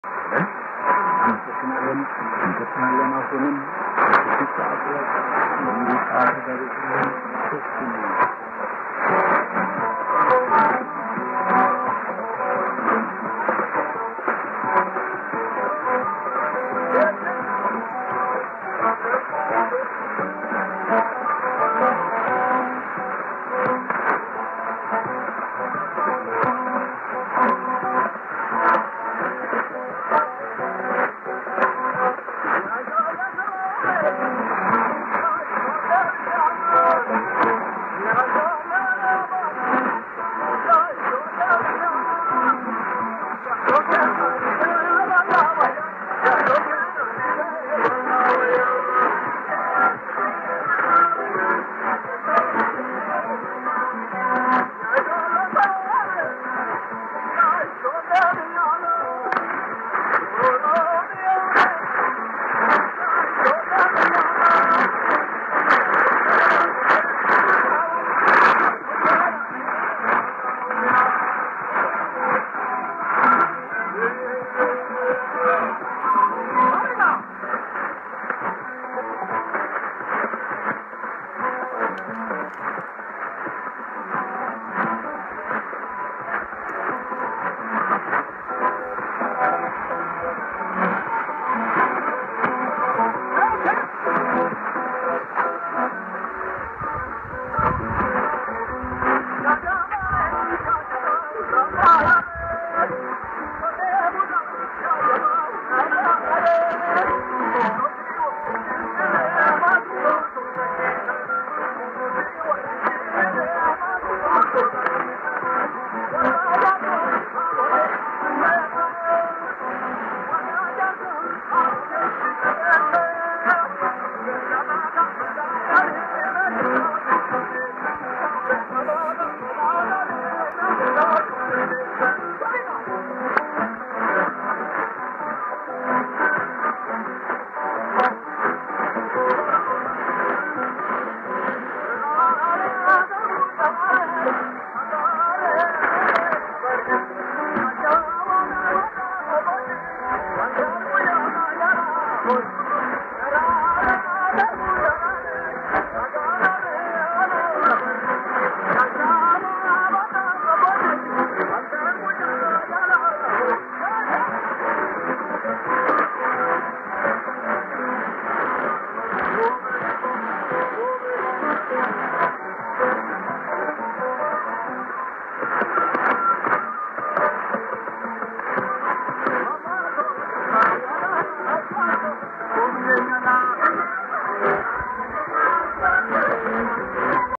DX'pedition: Radio Fana 6110 kHz Ethiopia, early evening reception
A decent signal with great music from Addis Ababa in Oxford UK on 27/04/16 at 19:30 hrs UTC using a Sony ICF-2001D receiver and non-resonant longwire. I was pleased with this early evening reception.